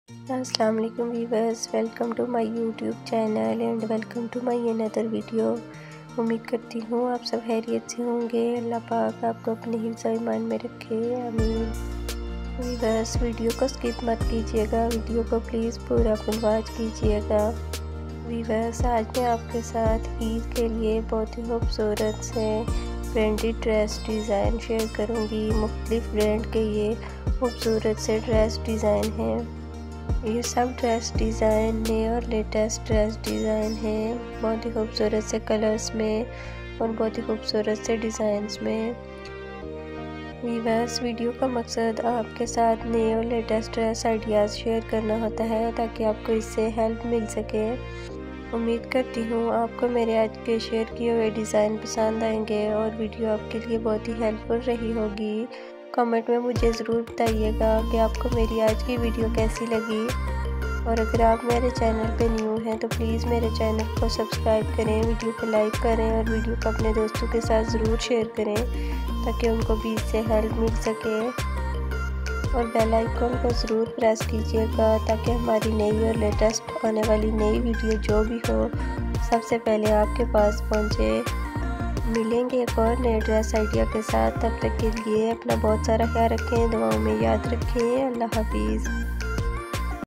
Branded dress design idea for girls||latest eid dress 2023||cotton
In this video you will find branded dress designs for Eid,In this video you will find the latest dress designs for Eid 2023,
The purpose of the video is to share the latest dress ideas with you so that you can get help from it.